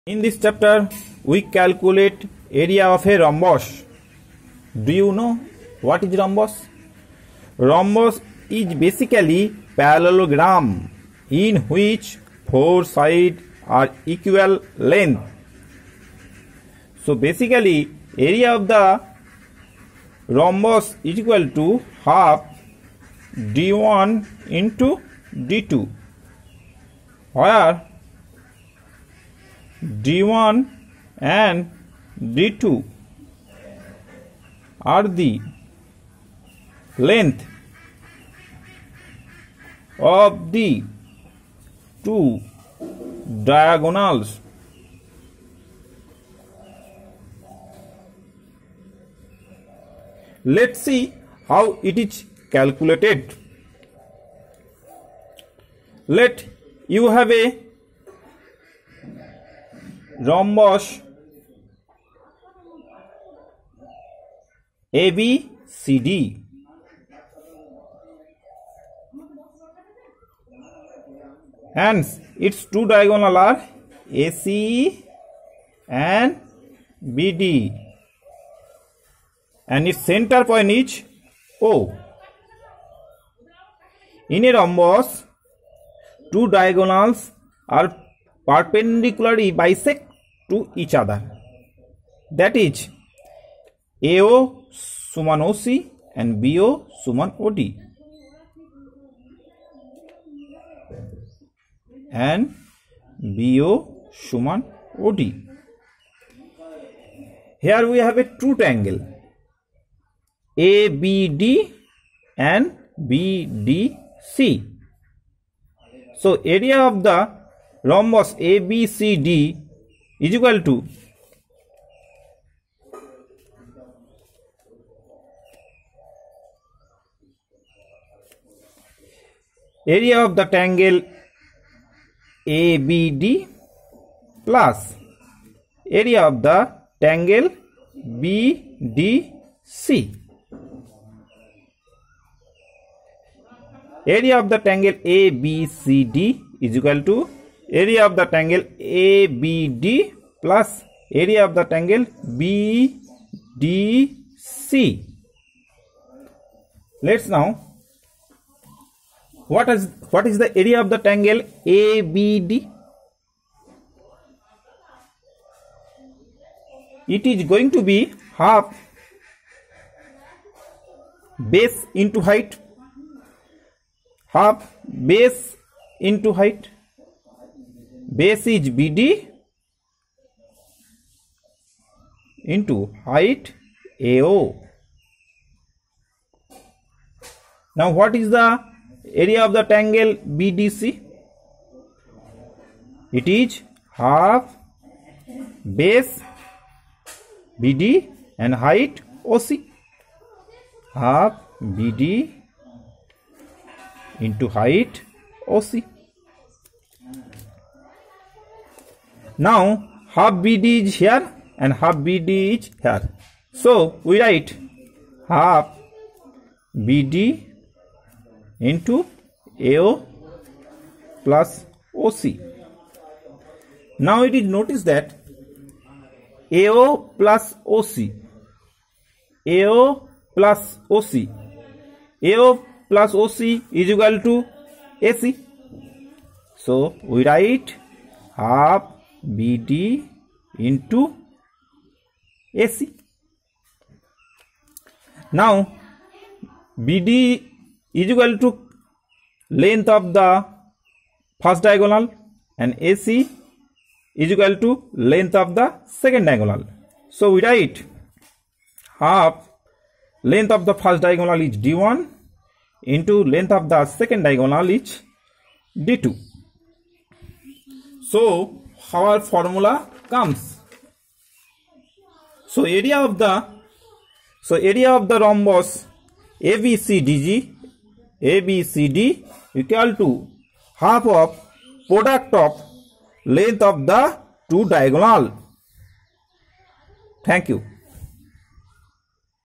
What is Rhombus | Formula For Area of Rhombus | Geometry Class | How to Find the Area of Rhombus
Here You Will Learn How To Calculate The Area of the Rhombus And Know the Formula of the Rhombus#RITUHOMETUTORIAL